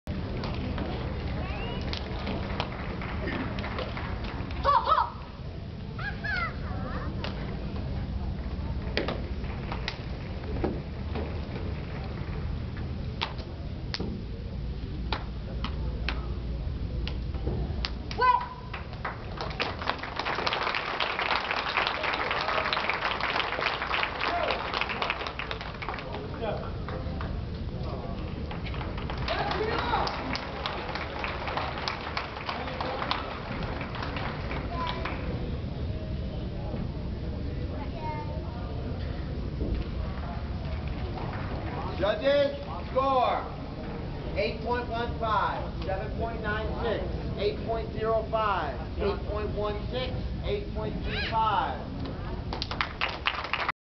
Keanan (nephew, sister Joi's son) Tong Bei Quan
Las Vegas Chinese Martial Arts Championship, August  2008. Currently training at Shaolin Kung Fu Chan, Las Vegas, Nevada.